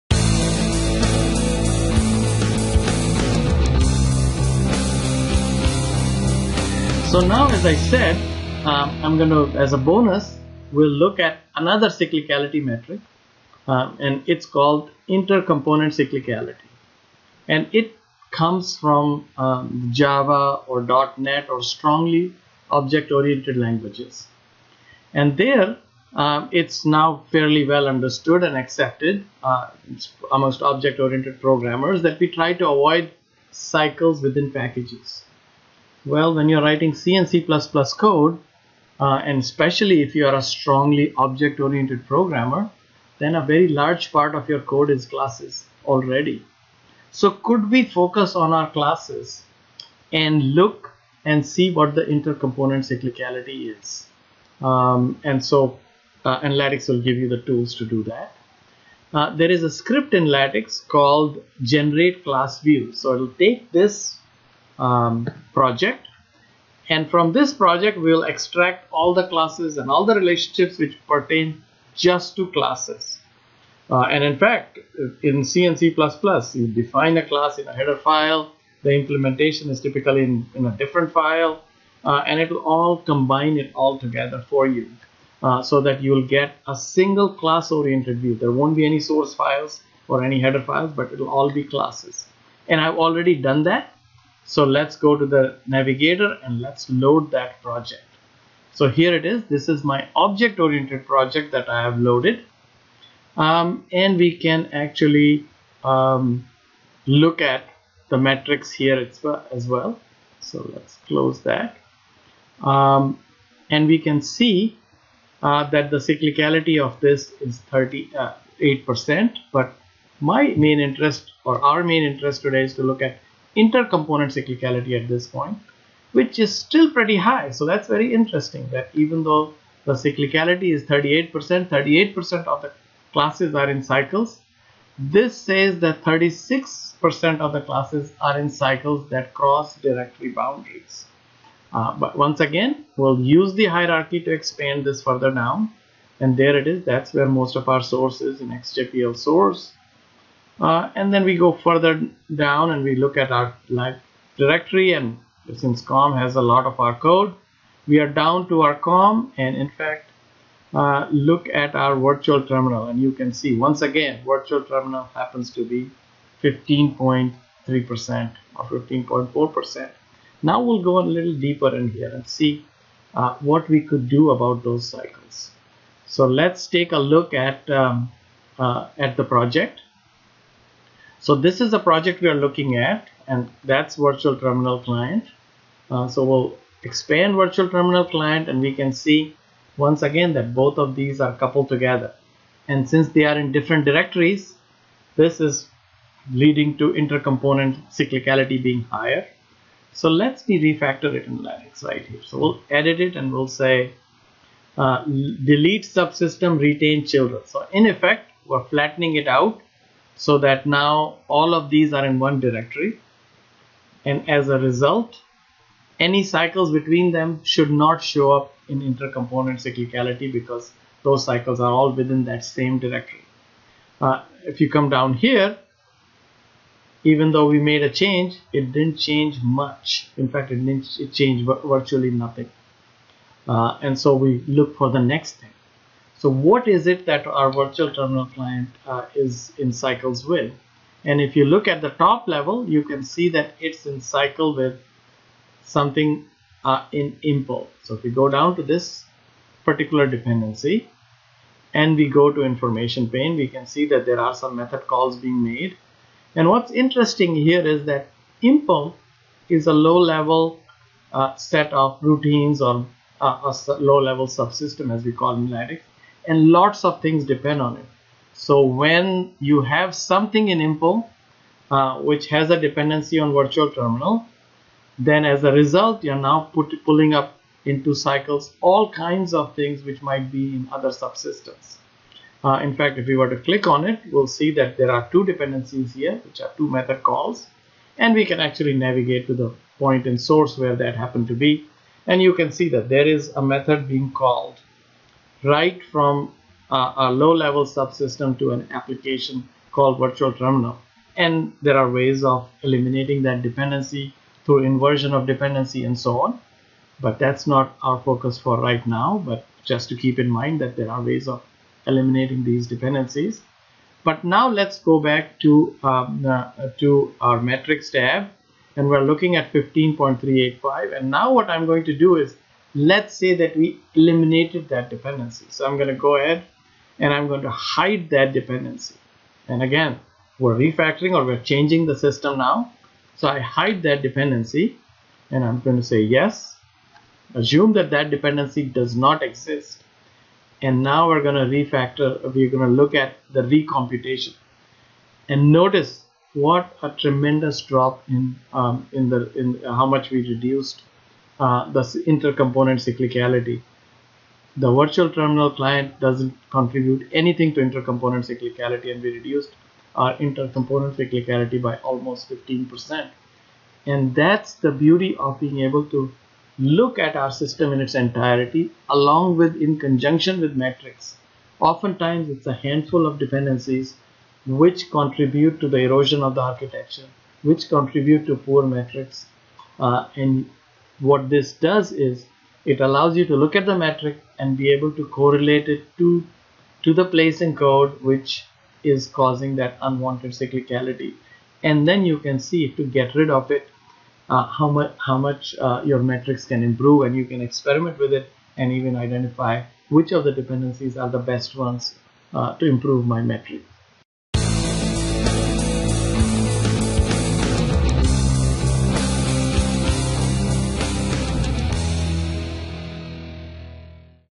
Modularity Metrics, Part 2: Intercomponent Cyclicality
Join Neeraj Sangal as he describes how to use metrics to identify how modular your software code base is. In Part 1, he looks in depth at the System Cyclicality metric. In Part 2, he looks at Intercomponent Cyclicality.